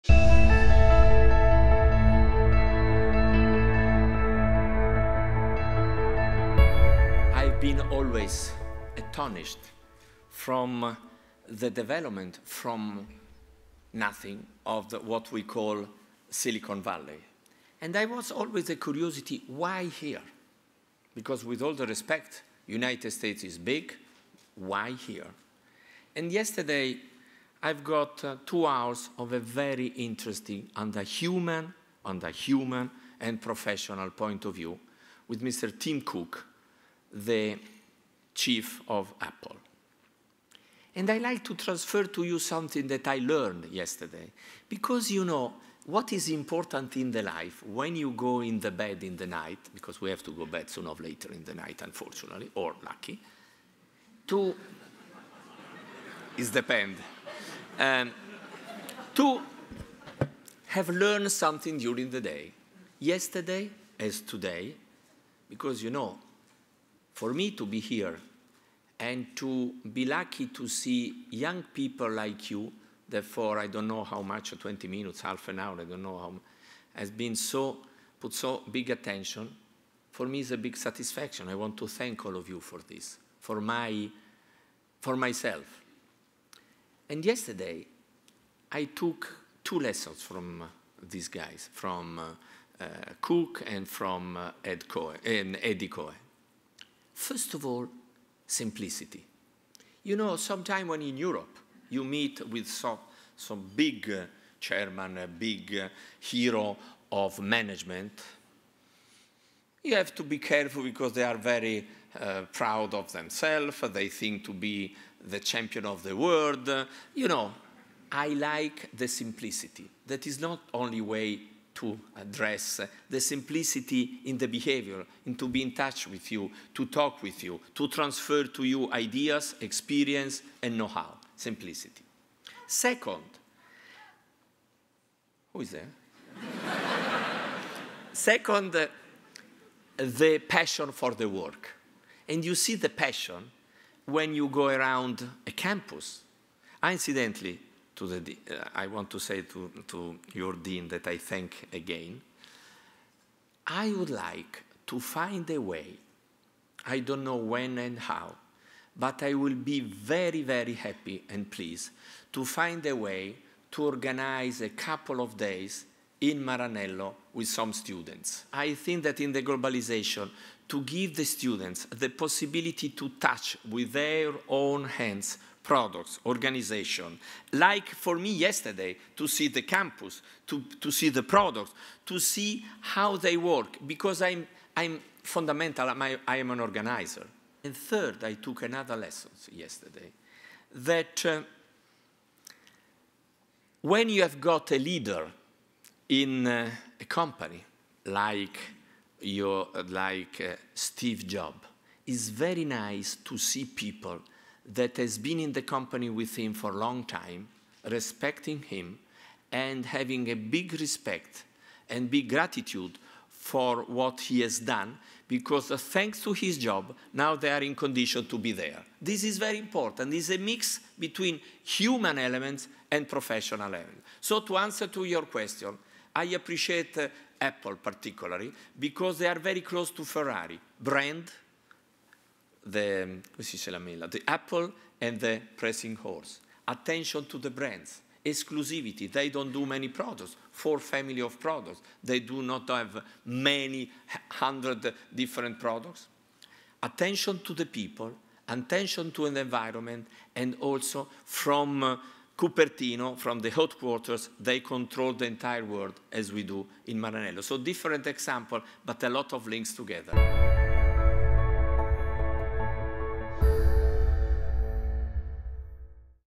Ferrari Chairman: Lessons from Silicon Valley
Ferrari's Luca Cordero di Montezemolo talks about what he considers special characteristics of Silicon Valley including simplicity, passion, attention to both people and the environment. He spoke April 24, 2012, at the Stanford Graduate School of Business' View From The Top series.